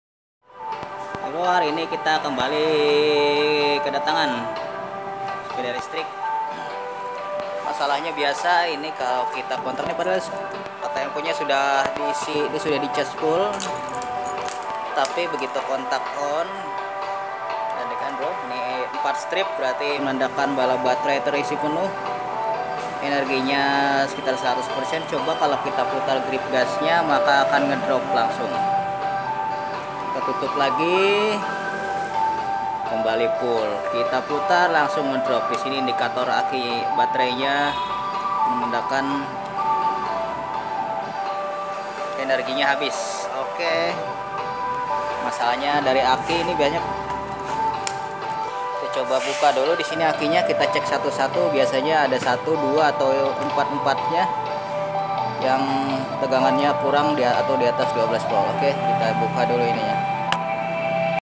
Servis sepeda listrik bermasalah pada battery
Servis sepeda listrik indikator battery penuh namun saat grip speed diputar langsung drop